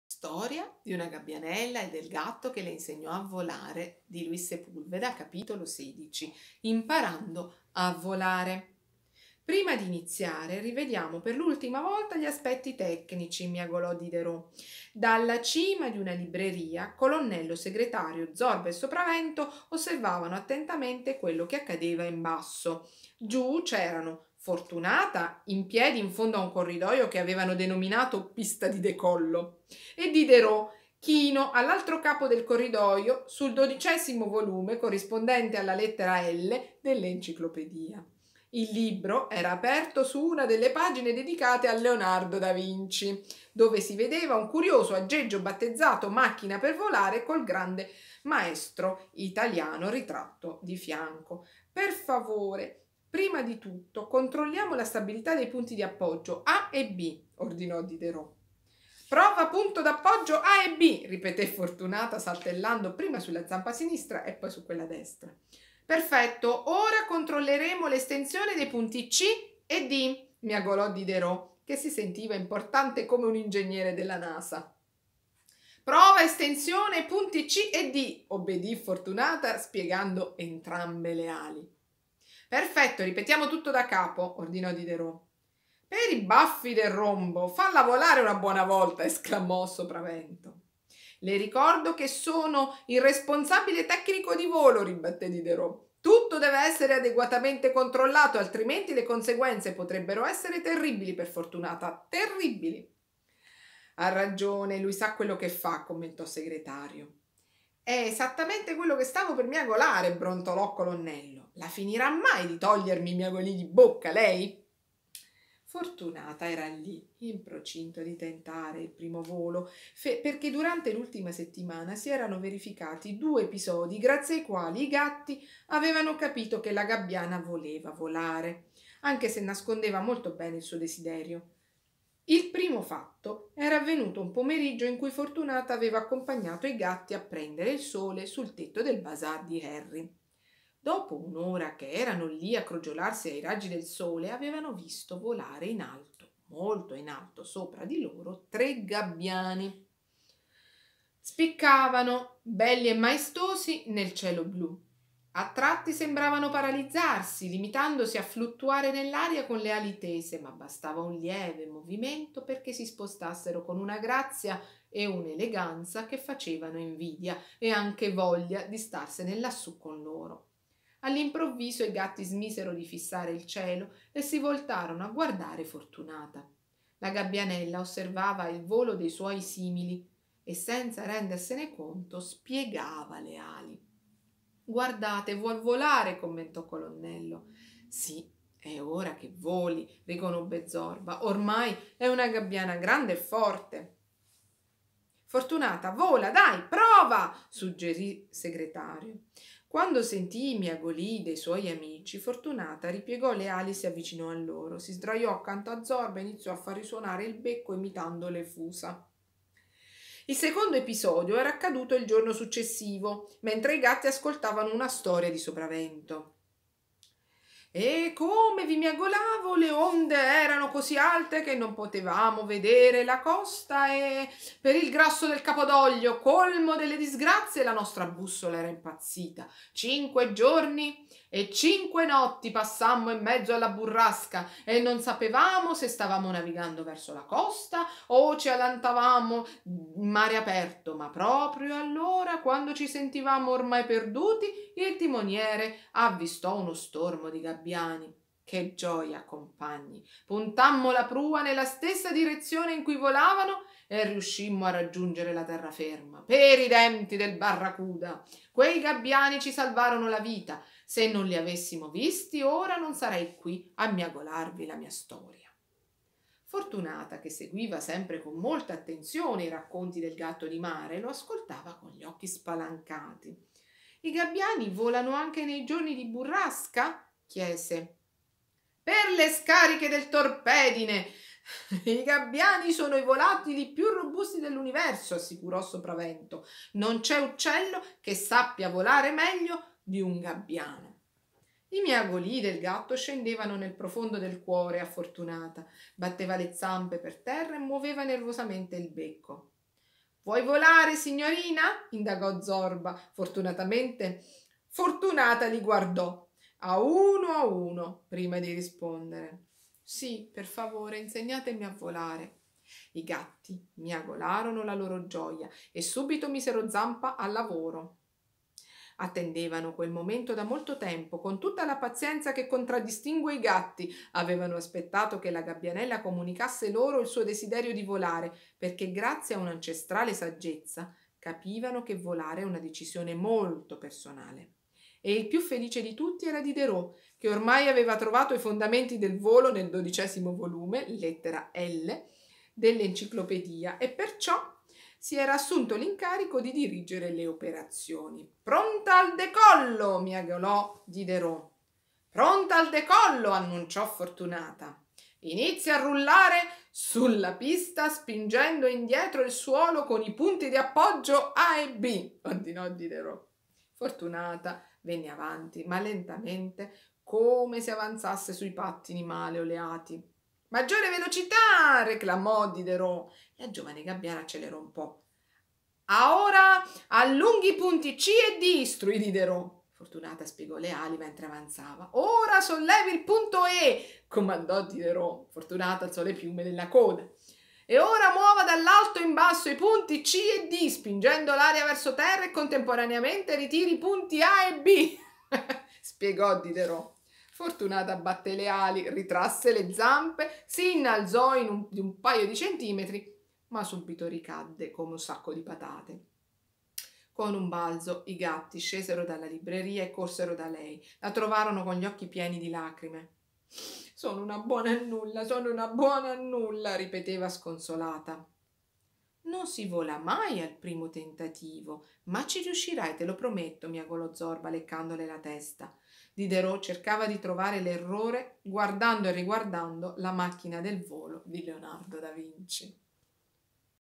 "Storia di una gabbianella (cap. 16) e del gatto che le insegnò a volare” di Luis Sepúlveda
tema: rispetto delle differenze, amicizia, impegno, superamento dei limiti, affermazione del sé, età 6+